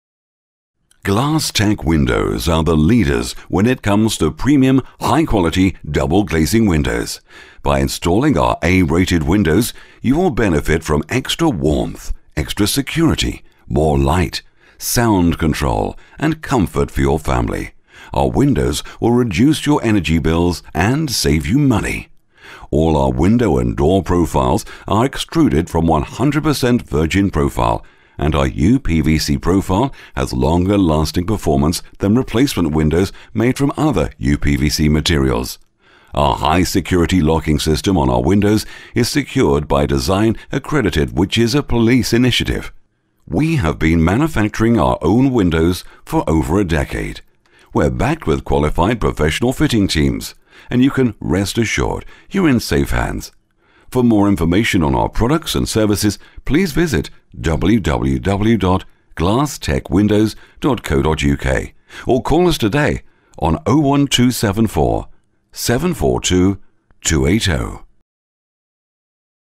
A Rated Windows by Glass Tec Windows (Bradford, West Yorkshire)
Today we have to be smart and be green, with our A Rated Energy Efficient windows you can choose a quality premium product backed by saving you money by reducing your energy bills.Your home loses heat through your windows . Installing energy efficiency windows is a proven and effective way of reducing your Energy bills.  Energy efficient double glazing windows also keep your home sound proof and warmer. for morw information on our products and services please

Glass Tec Windows
Unit 3, Harris Court
80 Beldon Road
Bradford
BD7 3DP